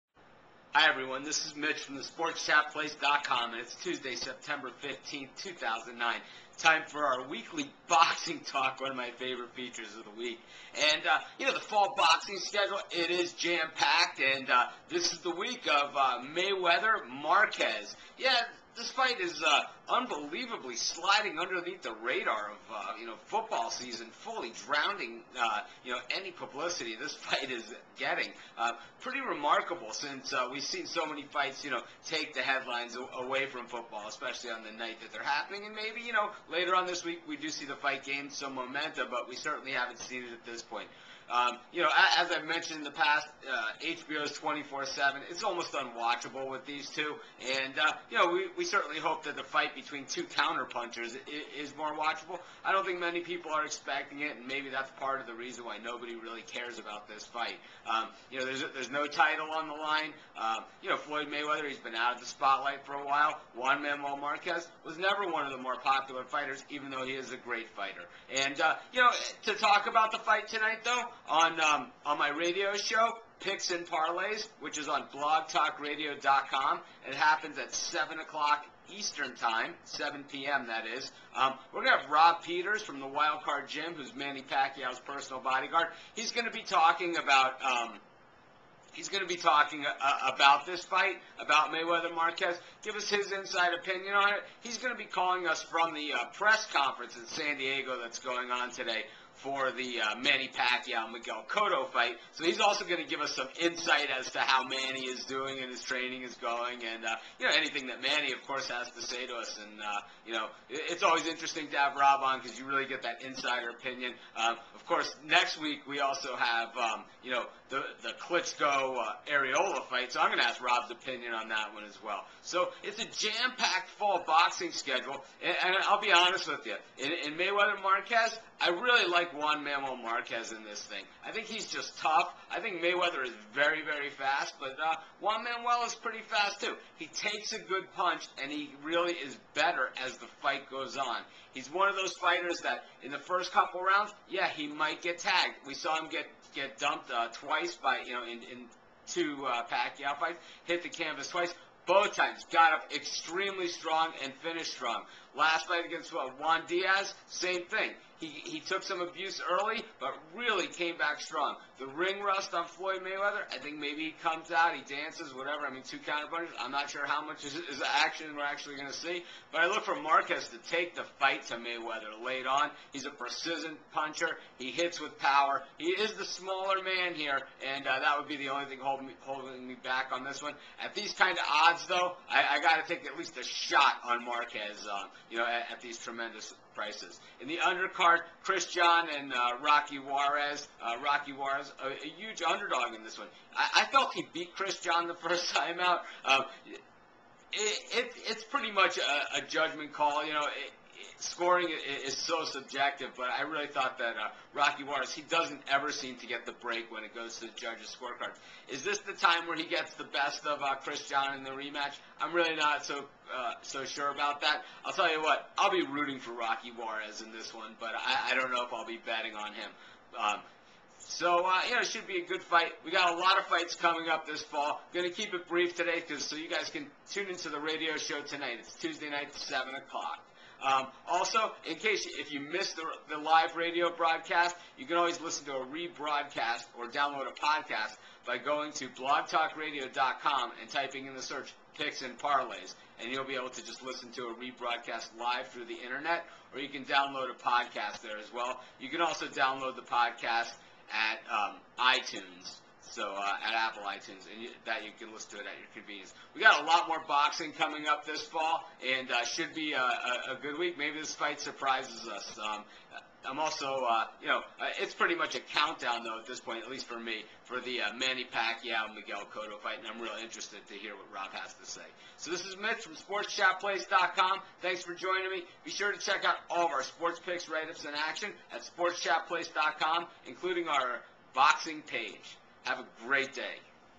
Boxing Talk 9-15-09 Mayweather, Marquez, Pacquiao, Cotto
Boxing Talk: Maywesther-Marquez, John-Juarez, Pacquiao-Cotto and more